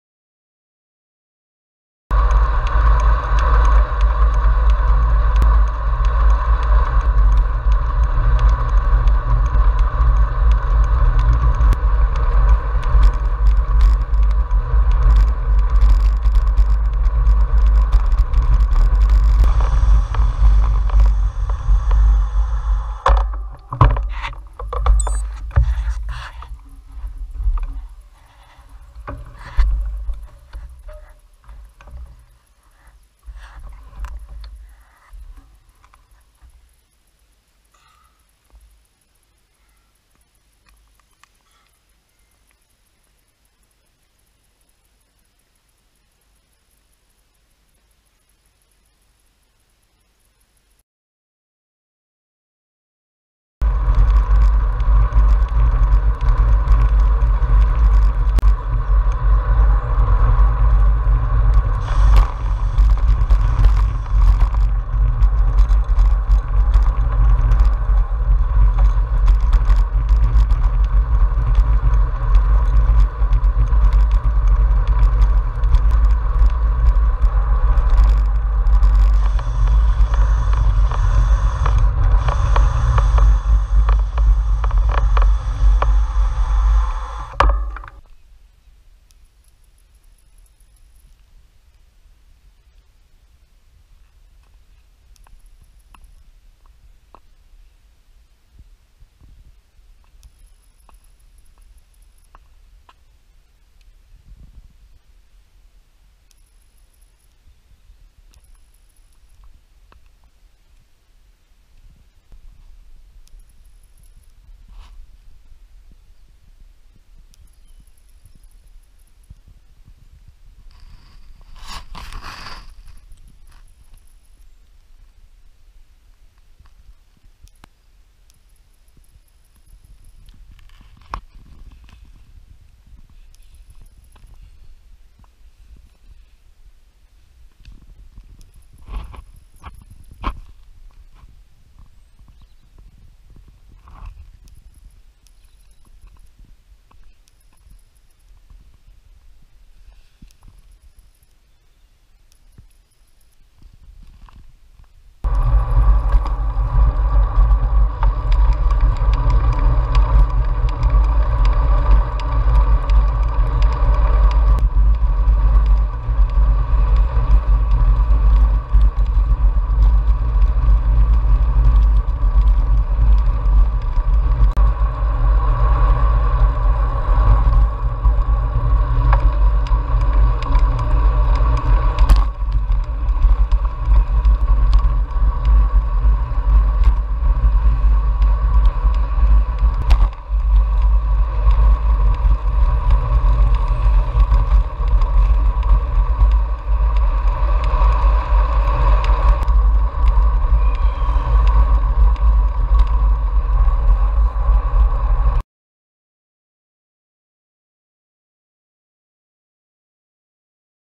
Cycling In Lincolnshire, England GoPro Sunrise
A short film of me sharing my ride back In the fens. Early morning ride watching the sunrise. Beautiful start to the day. The best time of day don't waste a second.